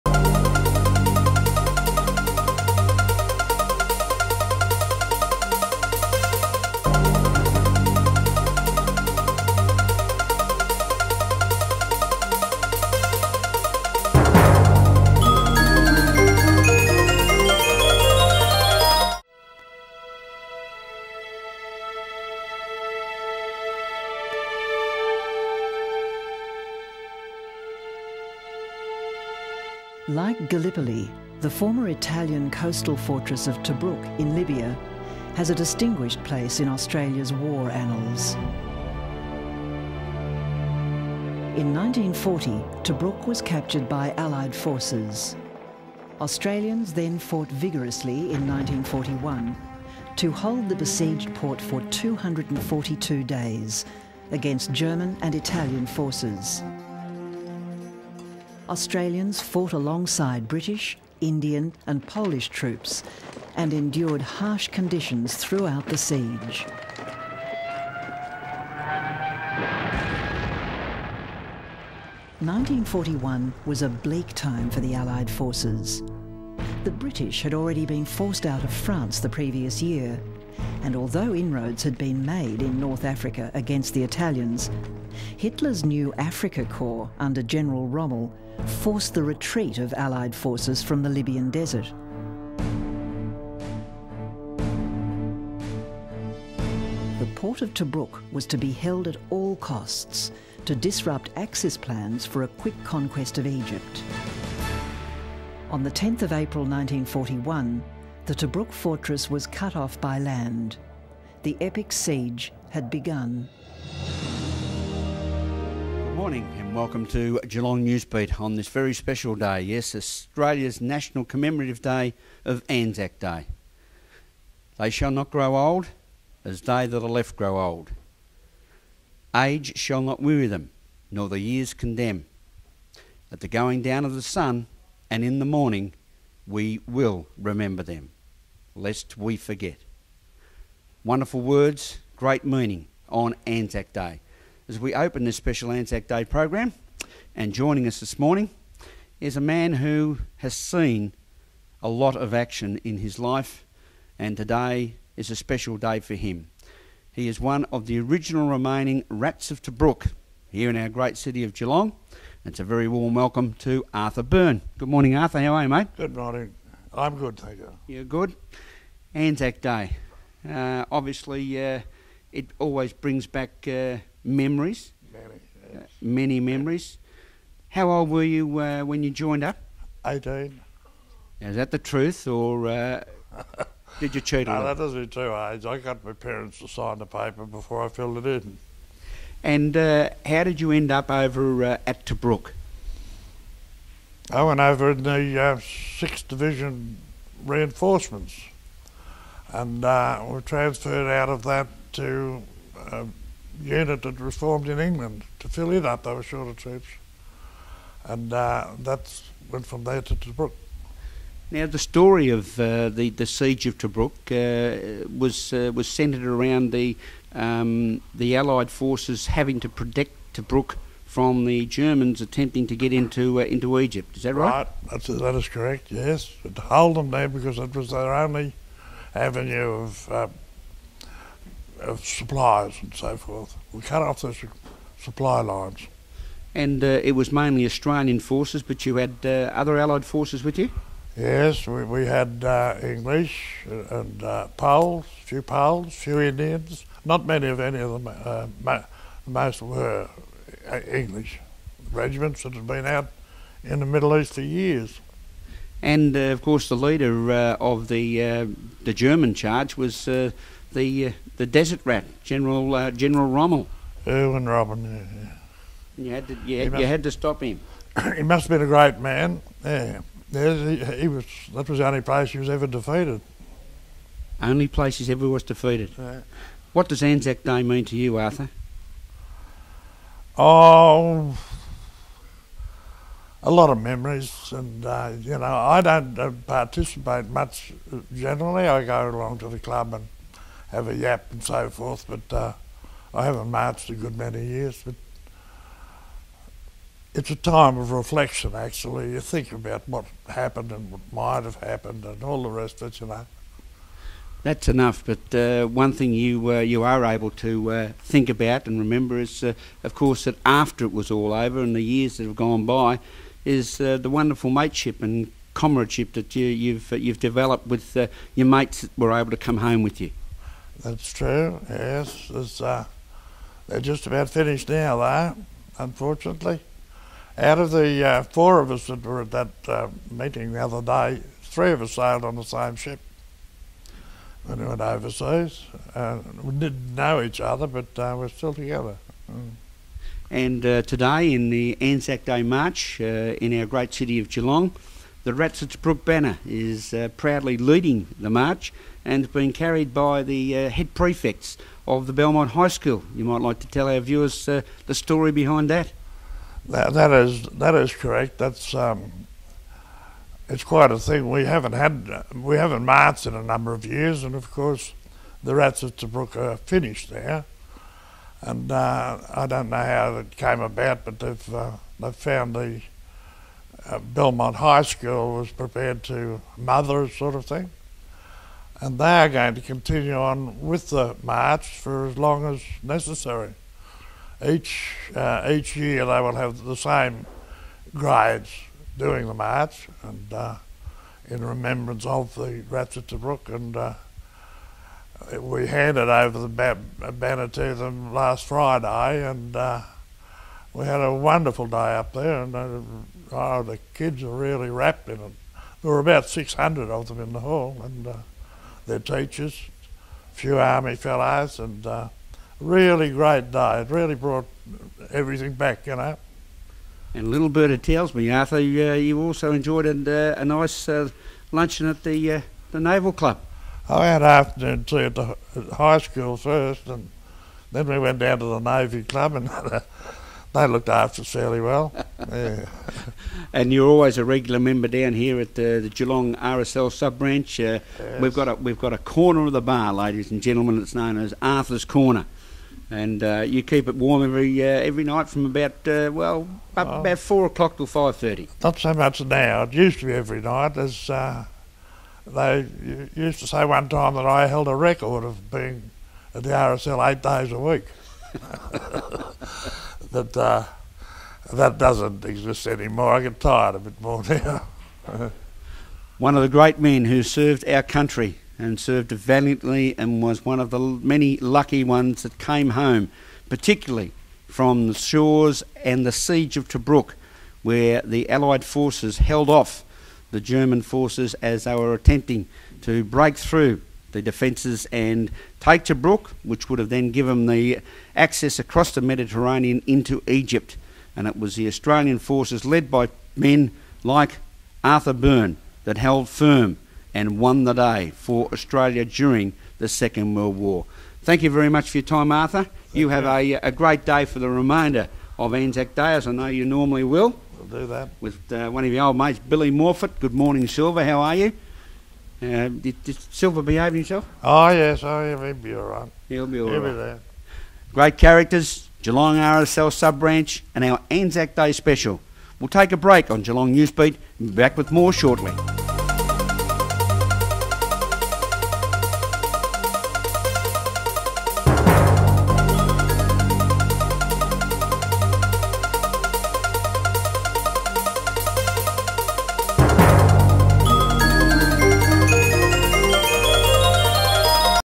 ANZAC Geelong
Graham Rawlins looks at Geelong's point of view for the ANZAC's